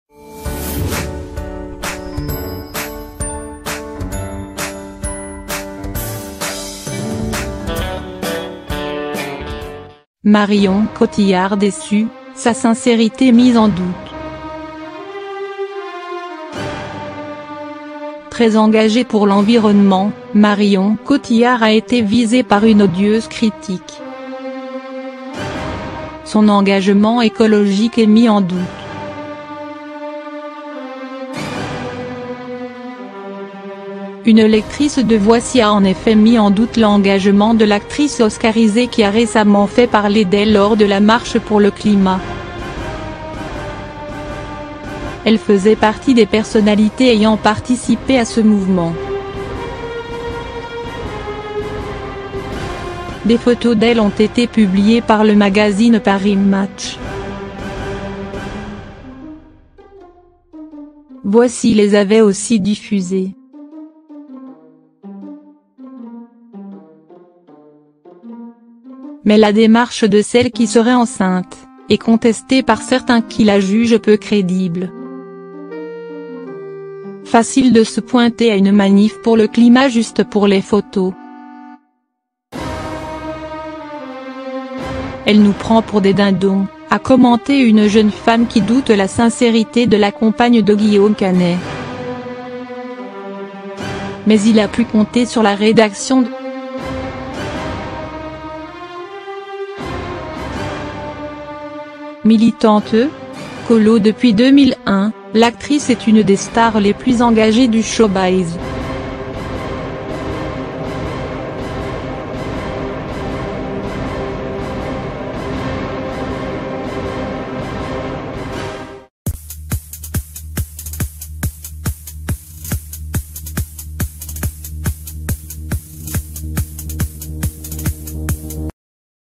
Marion Cotillard déçue, sa sincérité mise en doute
Très engagée pour l’environnement, Marion Cotillard a été visée par une odieuse critique. Son engagement écologique est mis en doute.